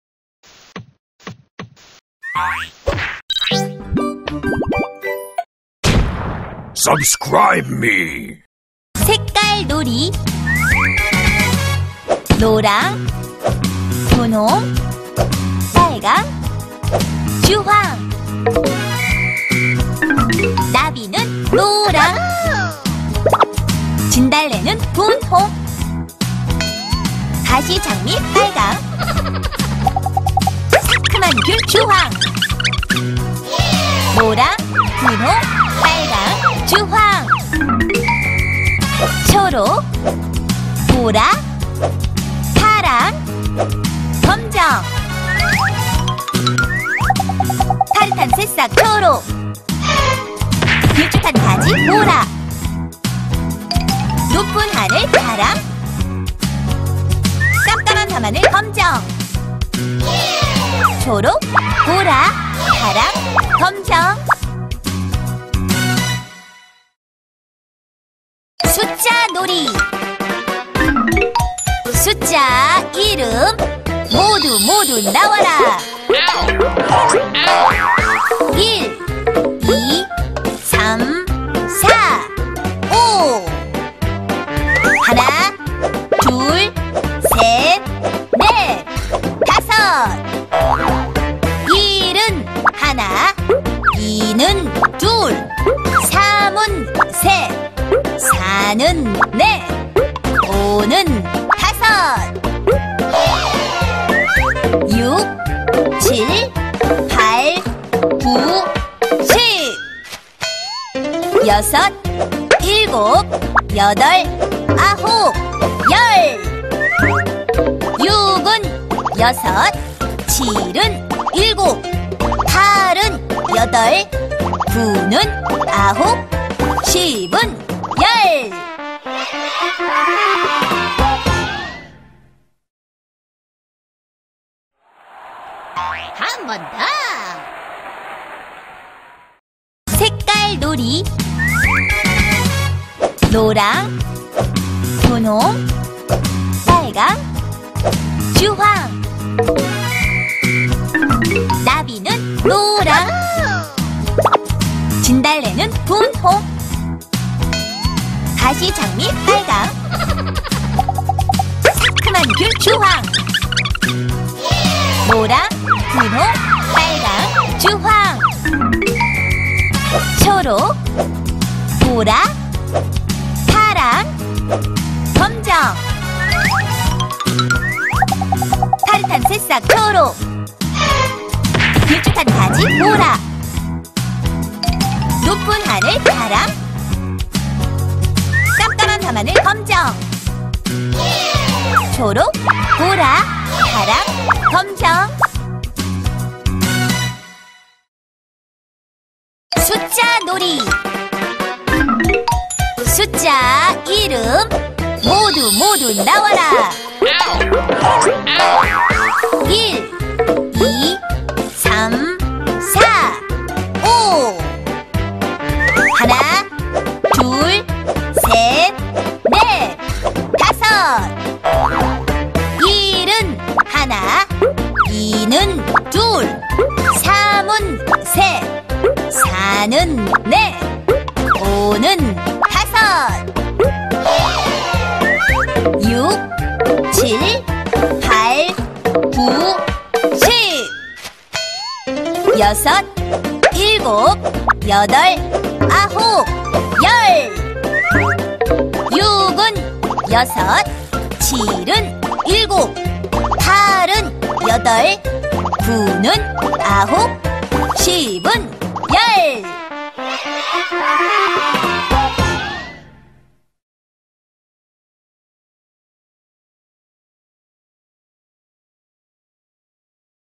단어 (색깔, 숫자) | 한글챈트 | 한글공부 | 한글놀이 | 깨비키즈 KEBIKIDS
색깔의 이름과 숫자의 이름을 챈트로 학습한다.